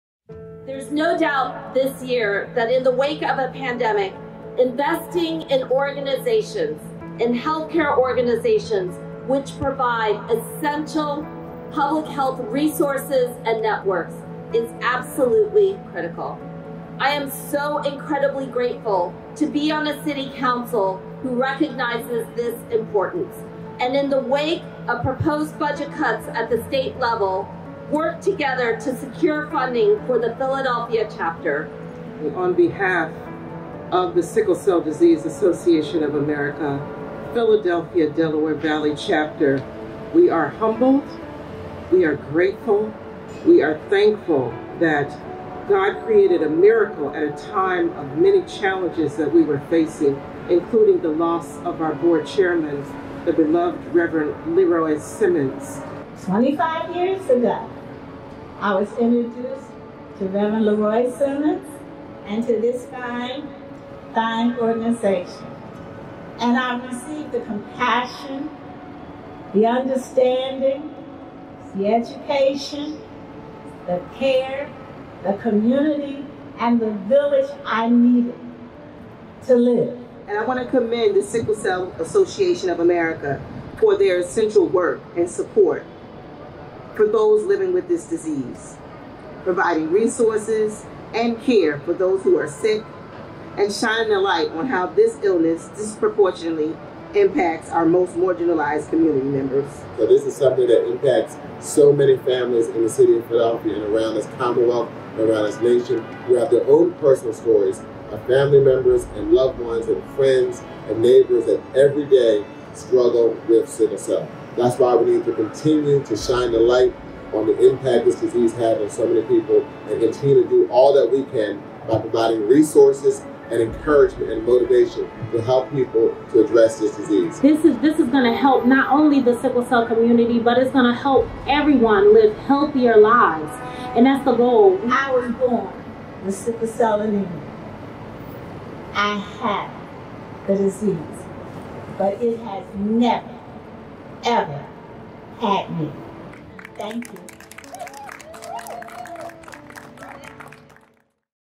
Shining a light on Sickle Cell
Councilmember Helen Gym joined Sickle Cell Disease survivors, the Sickle Cell Disease Association of America (SCDAA), and City Council colleagues to honor the SCDAA’s important work in providing culturally-competent services and reducing racial health disparities. In the wake of budget cuts during the pandemic, City Council secured $100,000 in funding for the Philadelphia/Delaware Valley Chapter of the SCDAA to ensure those affected by this serious disease would have continued access to their vital services, like counseling, transportation to appointments, and scholarships.